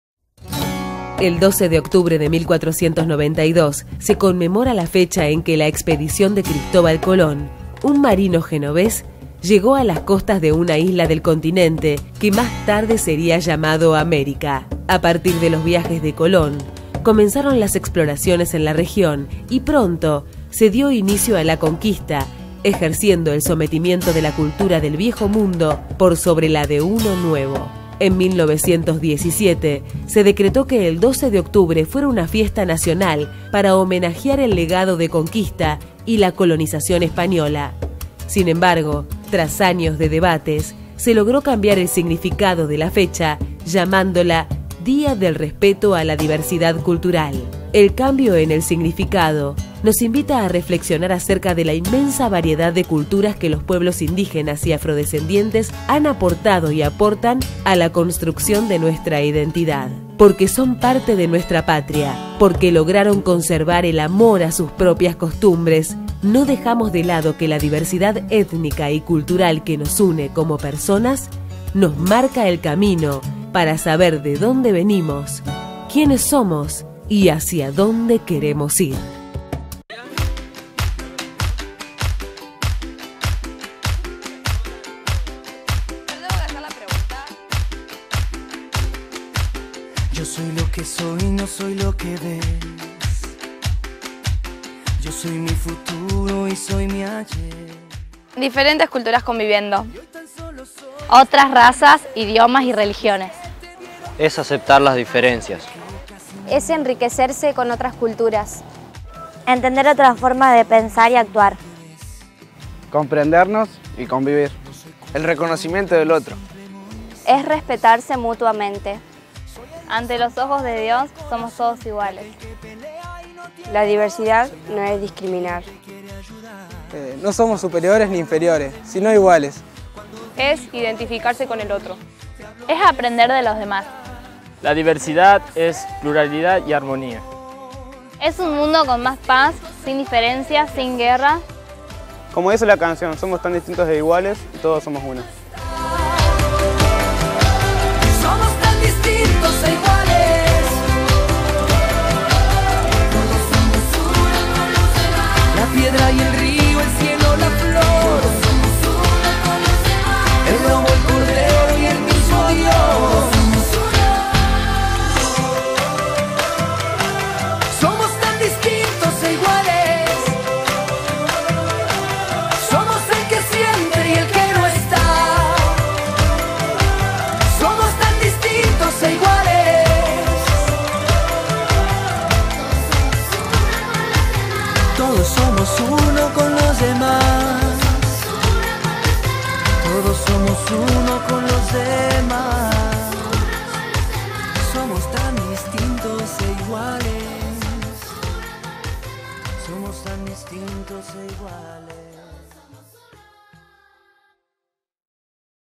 VIDEO 12 DE OCTUBRE DIA DEL RESPETO Y LA DIVERSIDAD CULTURAL
EspejoProducciones
Edición y Producción Prof Luciano Smut
Participación alumnos de 4to, 5to y 6to año   del IPEMYT 286 -Morteros-Córdoba- Argentina